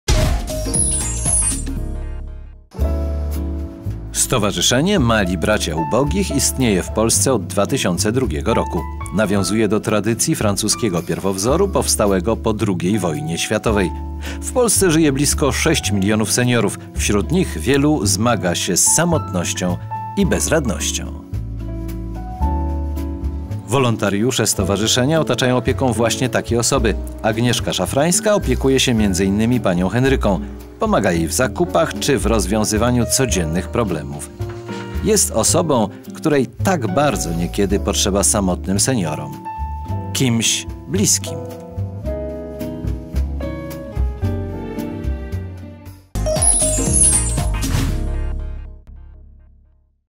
Mali bracia ubogich
Stowarzyszenie „Mali Bracia Ubogich"  otacza opieką samotnych seniorów.